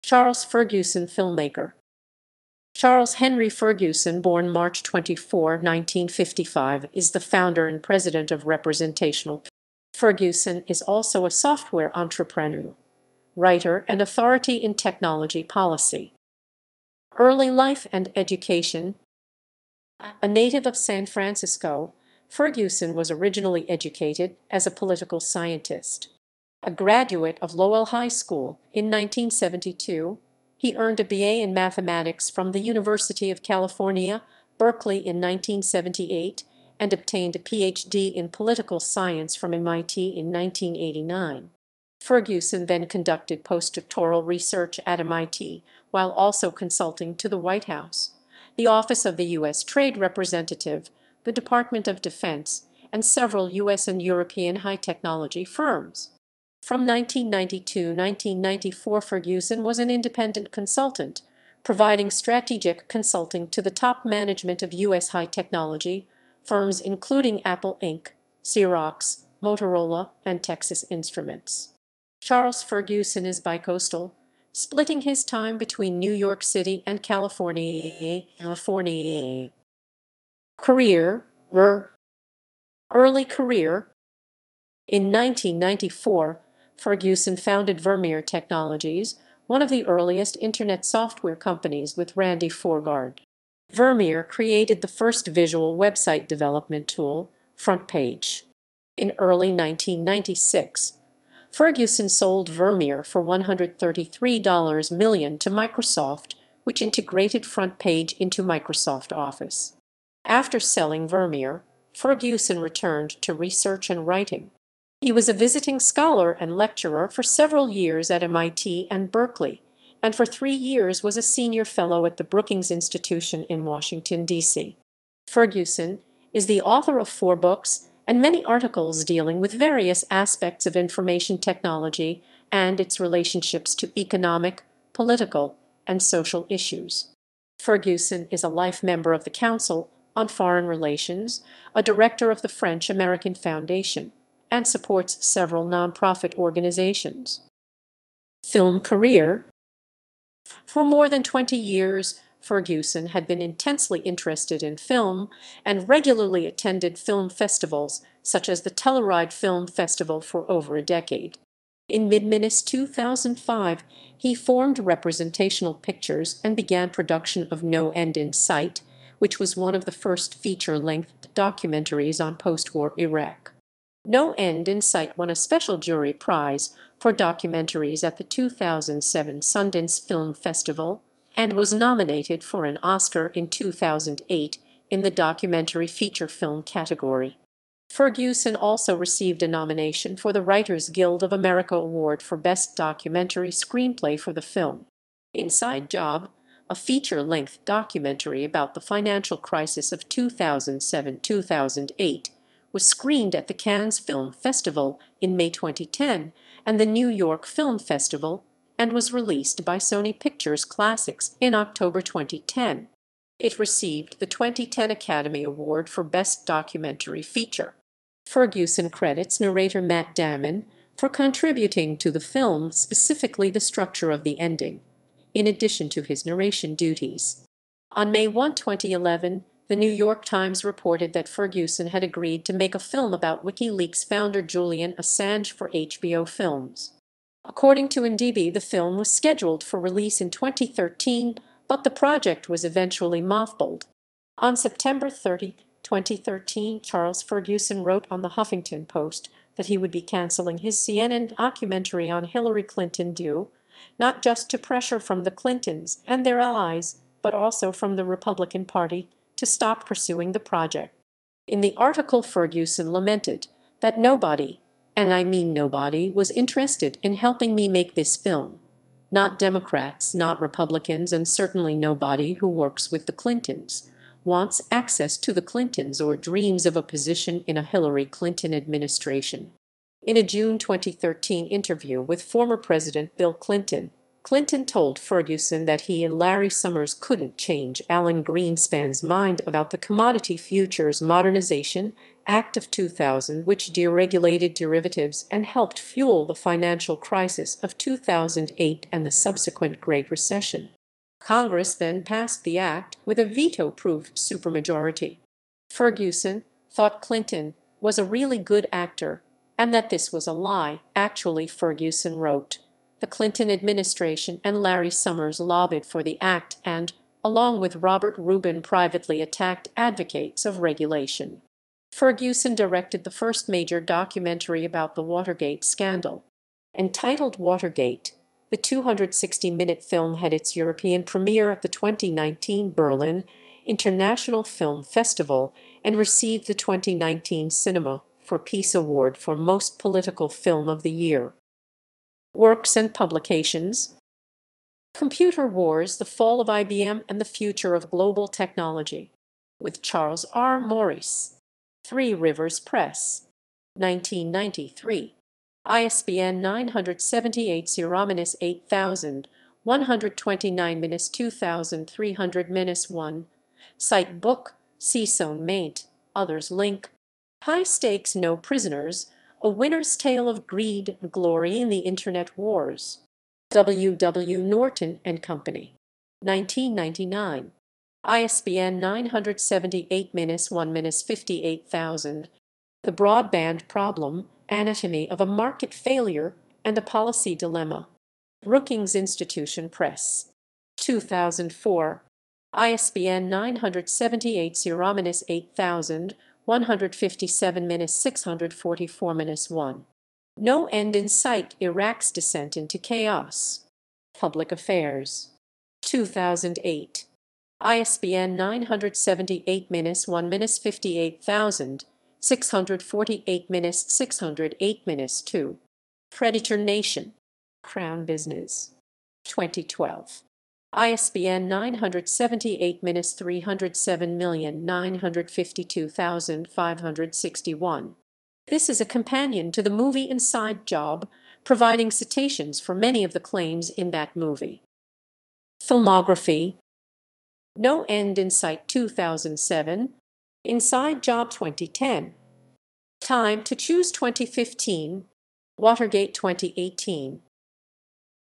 Charles Ferguson (filmmaker)
Charles Henry Ferguson (born March 24, 1955) is the founder and president of Representational Pictures, Inc., and director and producer of No End in Sight (2007) and Inside Job (2010), which won the Oscar for Best Documentary Feature. Ferguson is also a software entrepreneur, writer and authority in technology policy.